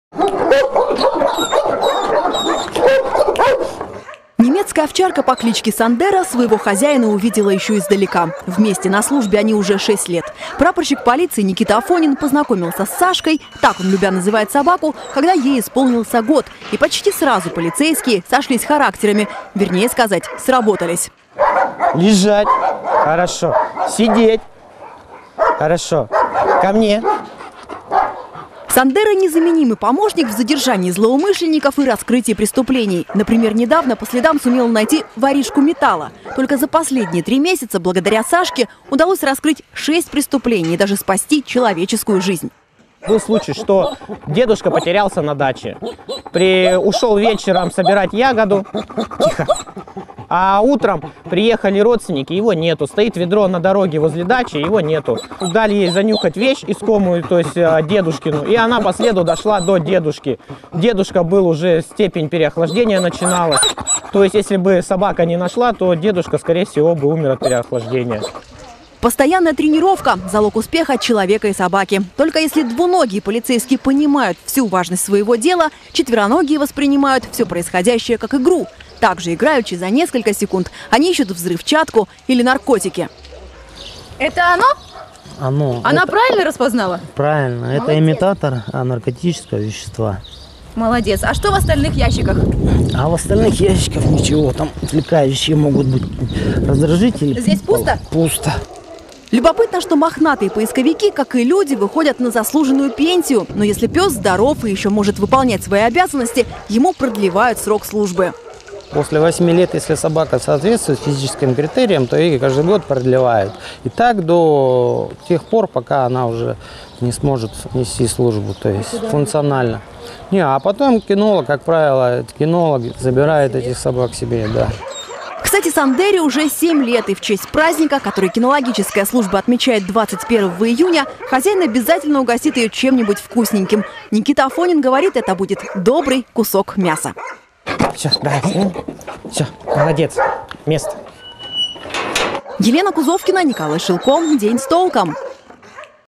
Как барнаульские полицейские и их питомцы учатся ловить преступников
В ближайшую пятницу кинологи и их лохматые помощники будут отмечать профессиональный праздник. Кинологической службе 110 лет. Ну, а сегодня наши корреспонденты понаблюдали за тем, как человек и собака готовятся к службе и ловят преступников. Рассказывает Елена Кузовкина.